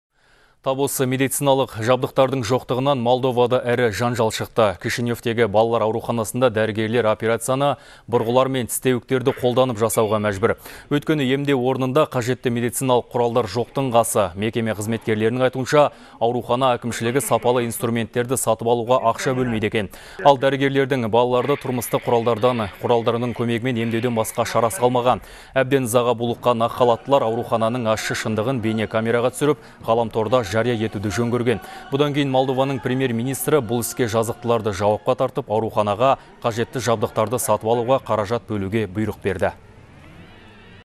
Молдовада дәрігерлер операцияны бұрғы және тістеуік көмегімен жасайды
Ызаға булыққан ақ халаттылар аурухананың ащы шындығын бейнекамераға түсіріп, ғаламторда жария етуді жөн көрген.